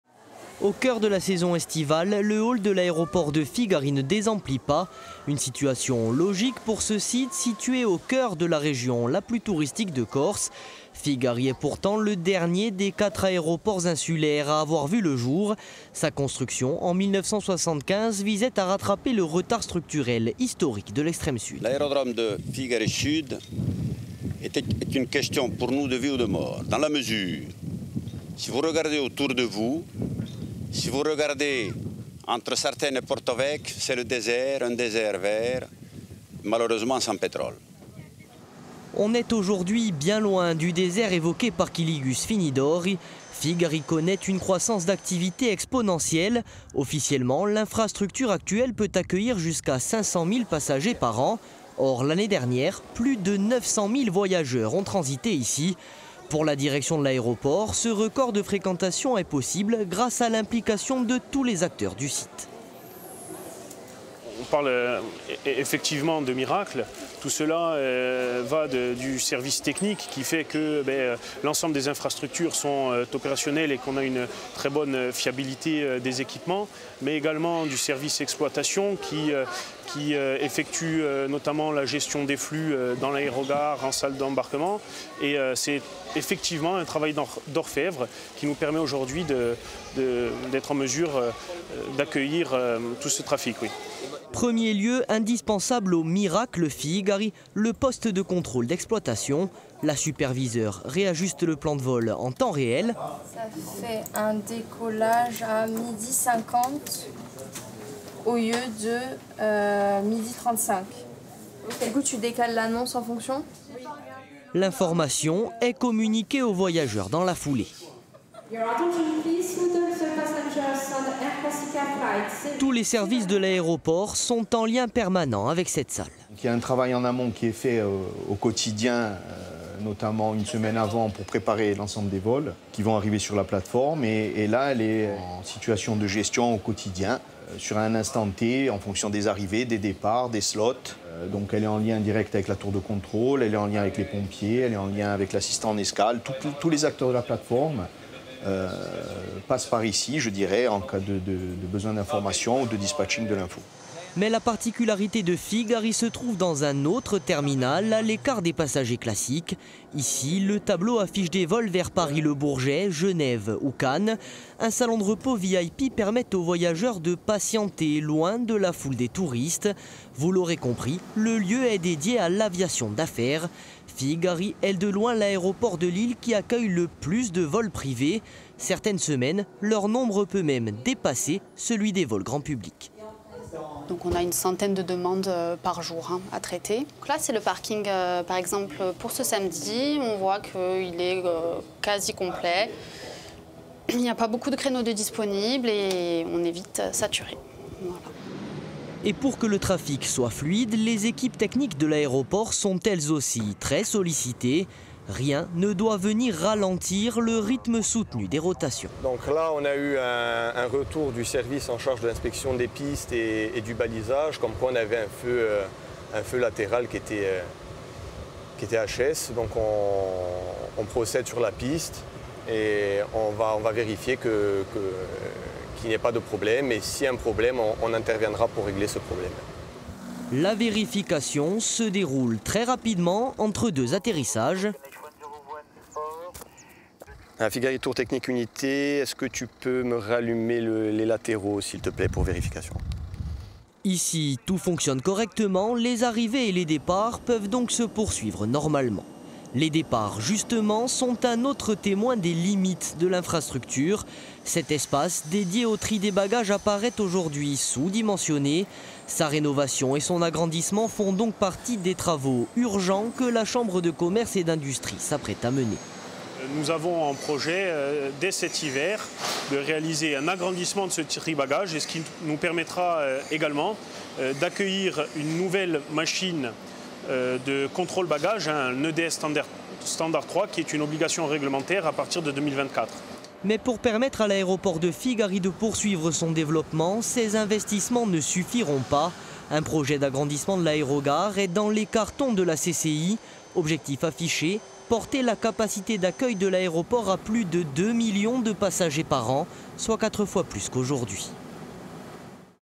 Série thématique : Ports et aéroports de Corse
Episode 5 : L’aéroport de Figari

L’aéroport de Figari Sud Corse connaît une croissance forte ces dernières années : le trafic passager y a doublé en 10 ans.
Aujourd'hui, les capacités d'accueil de l'infrastructure sont largement dépassées. Nous nous plongeons dans les coulisses du fonctionnement de ce site stratégique, au cœur du territoire le plus touristique de notre île